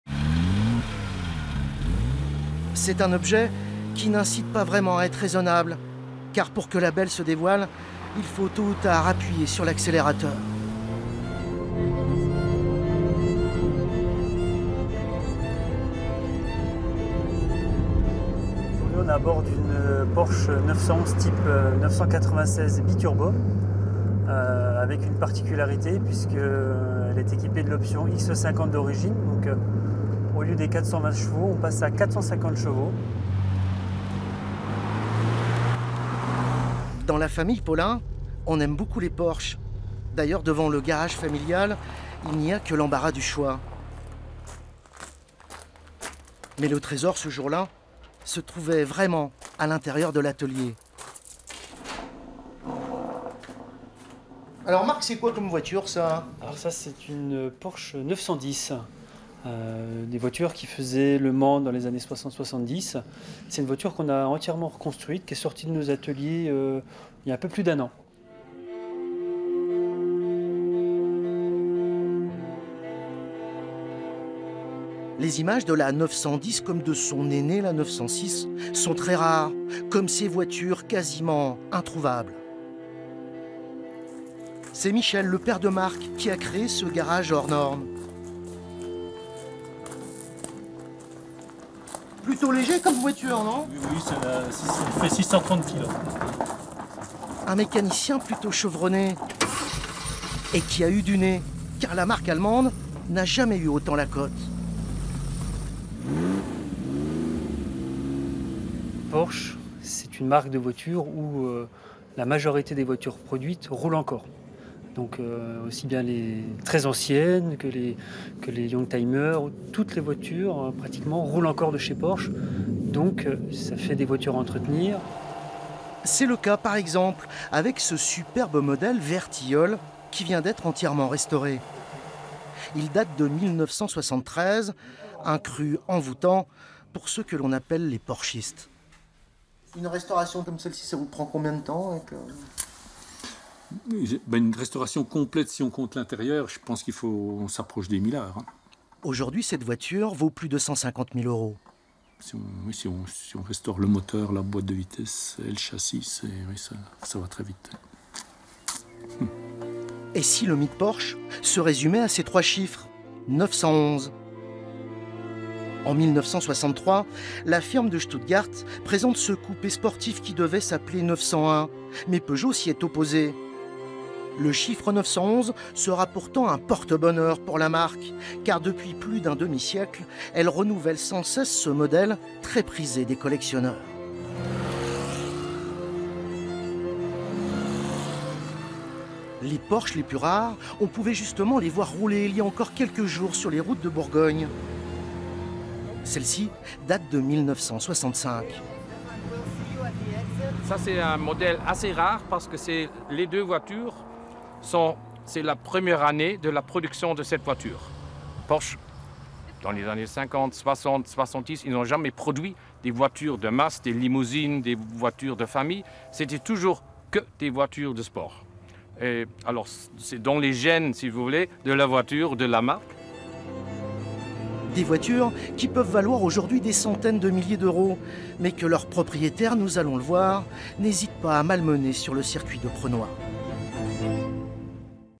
Feuilleton : les fans de voitures Porsche 1/4
Le feuilleton cette semaine est consacré aux porchistes. Ce terme désigne  les passionnés de la marque Porsche et dans notre région Ils sont nombreux  à vouer un culte immodéré  aux  voitures  de la firme de Stuttgart et notamment à la Porsche 911 qui existe  depuis le milieu des années 60 !  On peut même parler d'un véritable  phénomène de société. Nous allons voir que certains ont fait de cette passion un métier ..Direction Charnay dans le Doubs où se trouve l'un des spécialistes de ces bolides... bien évidemment inaccessibles à la plupart d'entre nous .

Reportage L. Ducrozet, D.Martin, J.Paulin, M.Dubuis
avec
Marc Paulin
Spécialiste de la marque Porsche
Michel Paulin
Fondateur de MJ Technic
Stephan Köning
Pilote allemand au Tour Auto Optic 2000